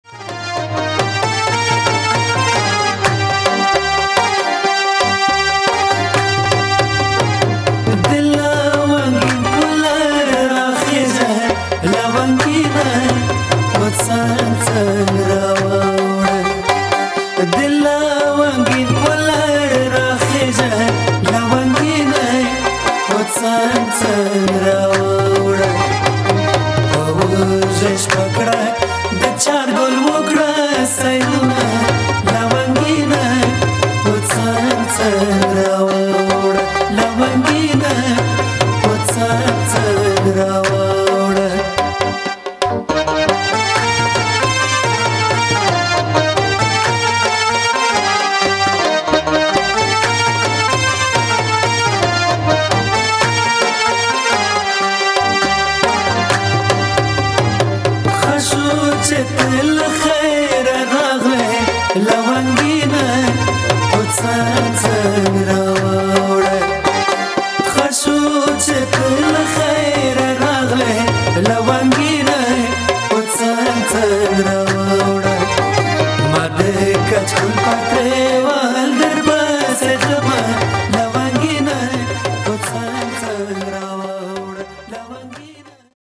Attan Nowroz 1388
Nowroz1388 Ceremonial Party organized by Katib Cultural Association. March 28th 2009 Copenhagen.